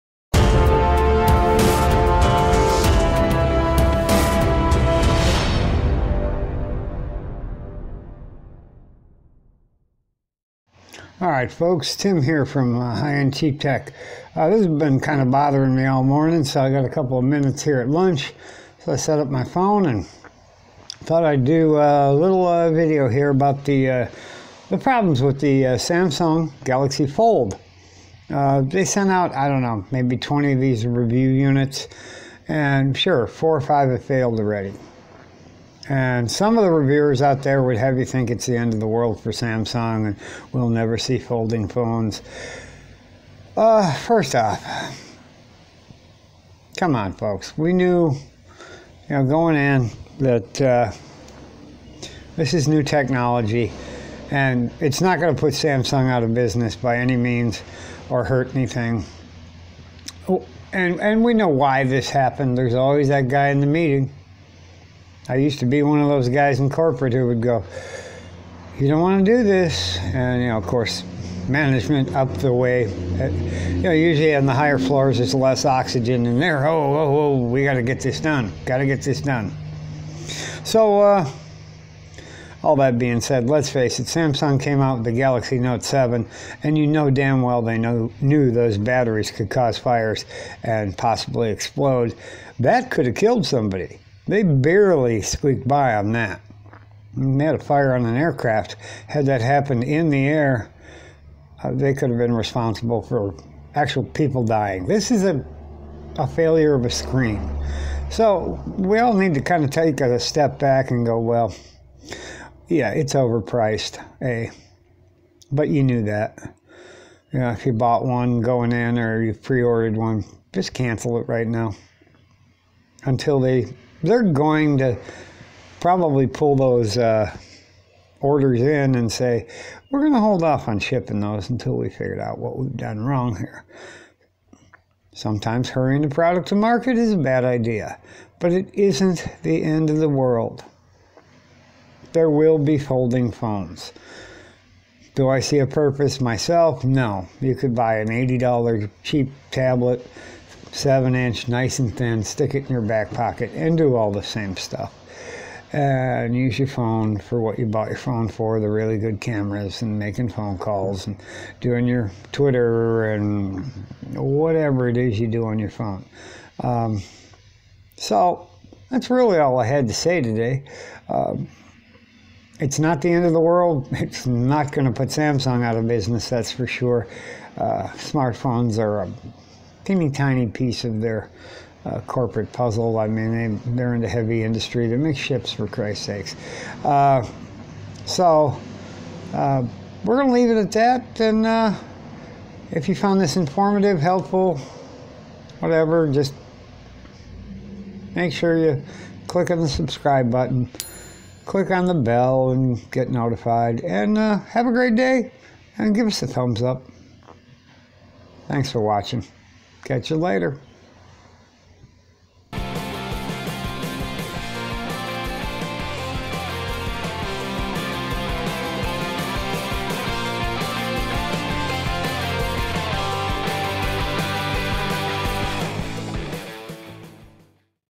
Galaxy Fold Screen Failures | The End Of The World?
Today a short video on the Galaxy Fold screen failure issues. Is it the end of folding phones? Of course not. So watch the video and learn something and leave some feedback.